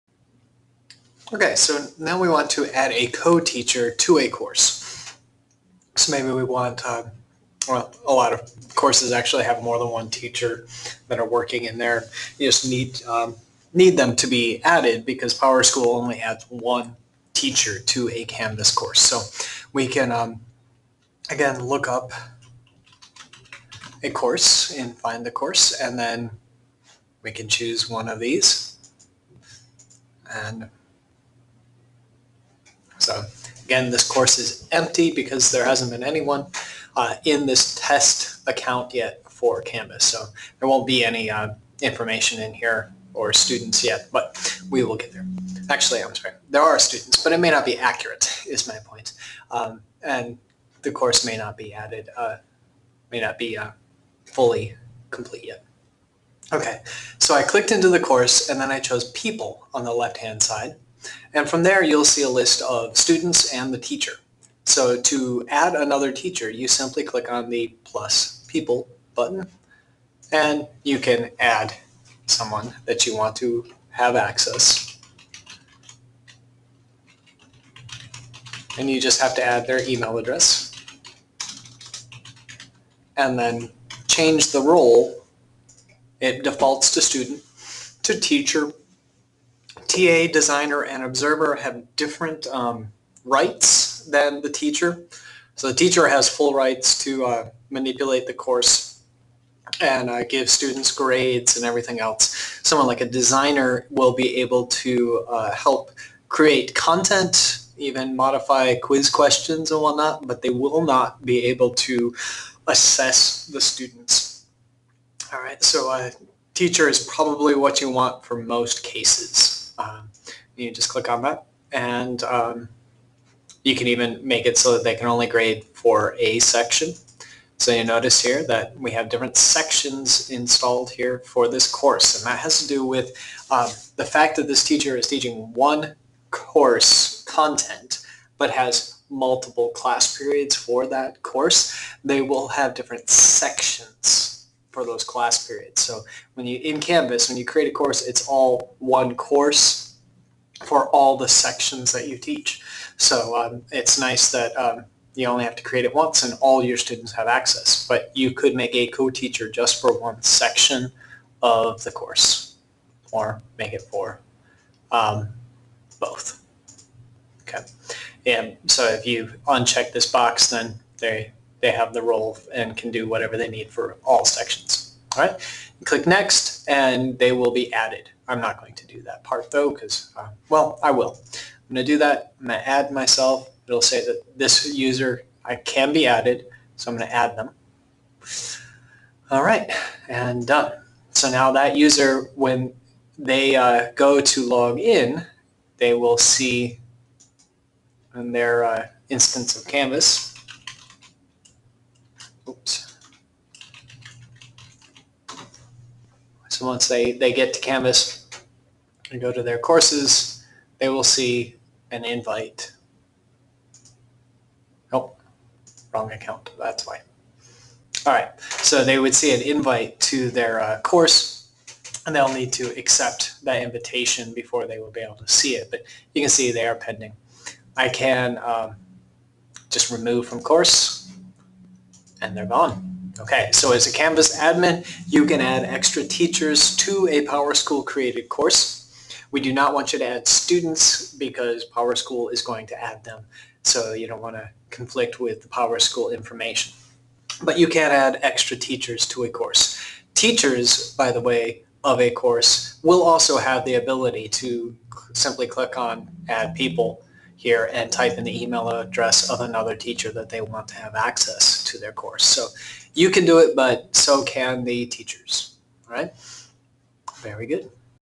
Add Co-Teacher To A Course
How to add a co-teacher to a Canvas Course.